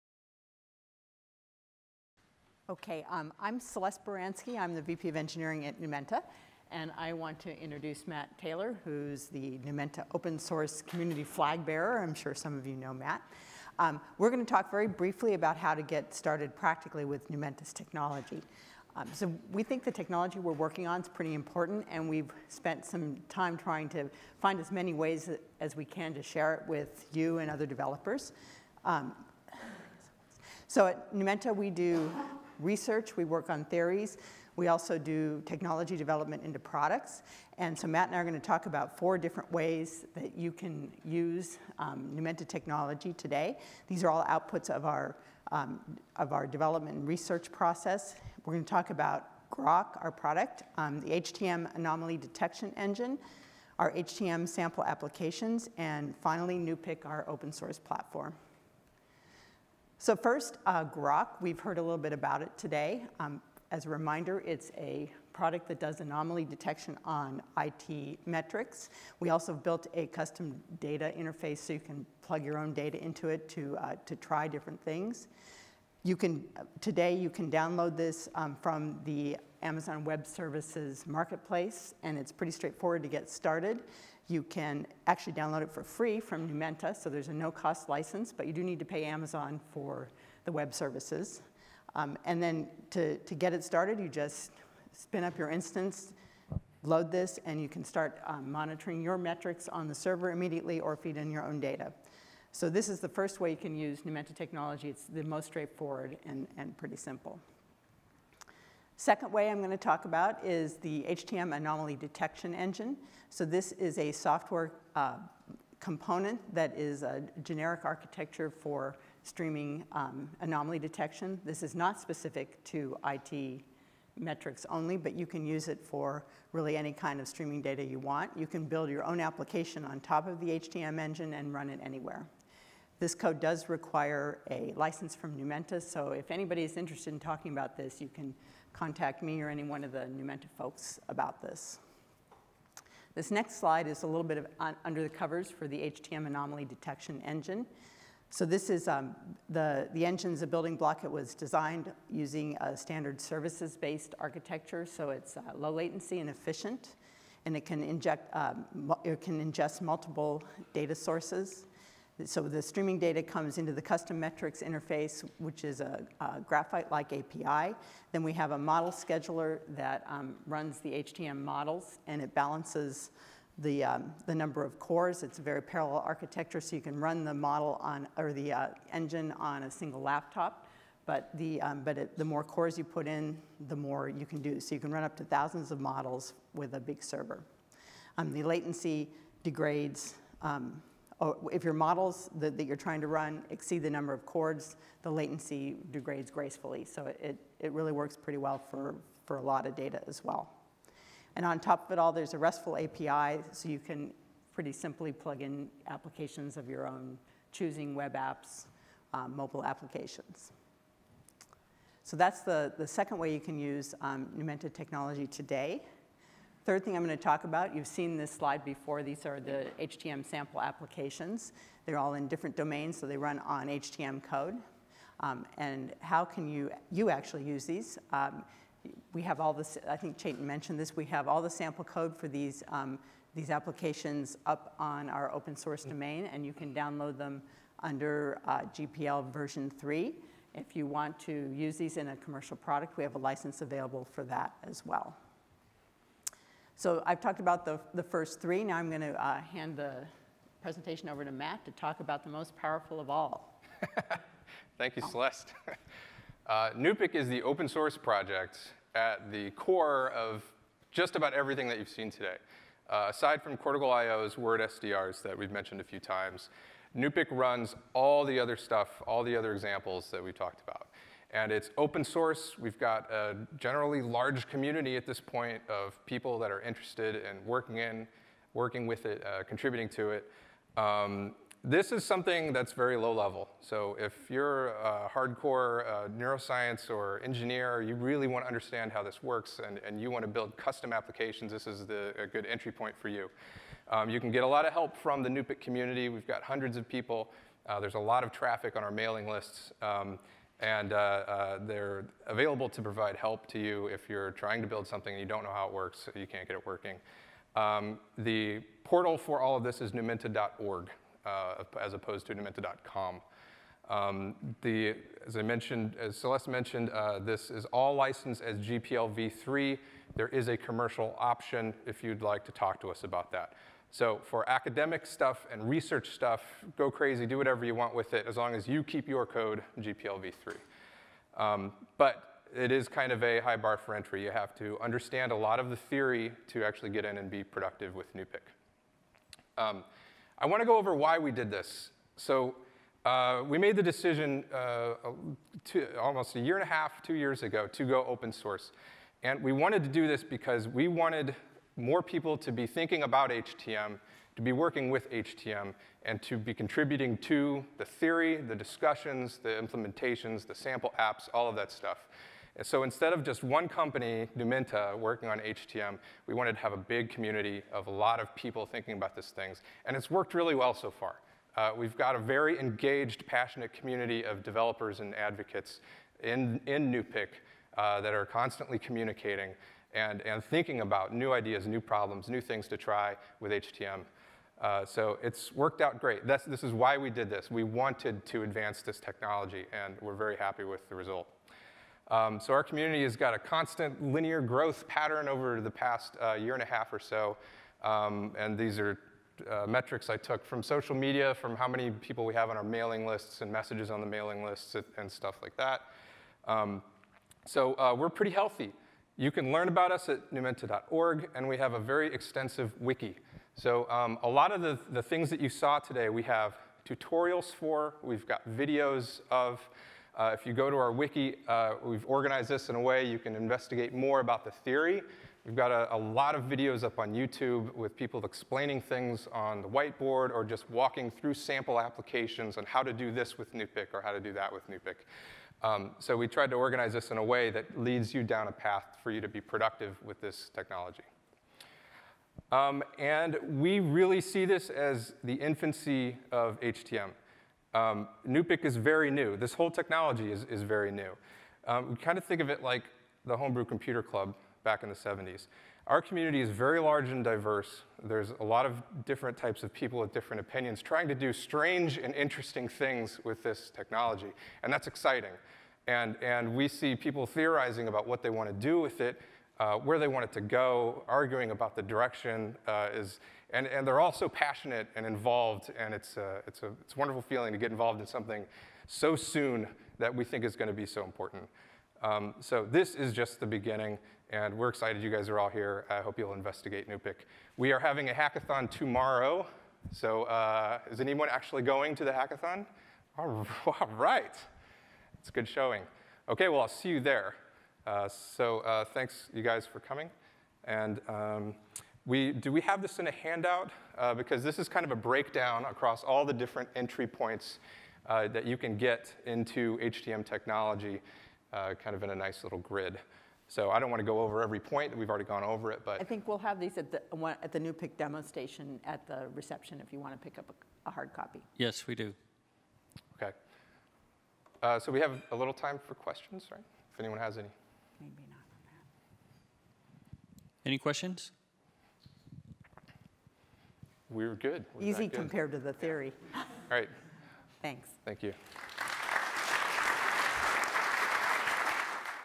Getting Started With Numenta Technology
Celeste Baranski, VP Engineering, Numenta.
Matt Taylor, Open Source Community Flag-Bearer, Numenta.

Numenta Workshop Oct 2014 Redwood City CA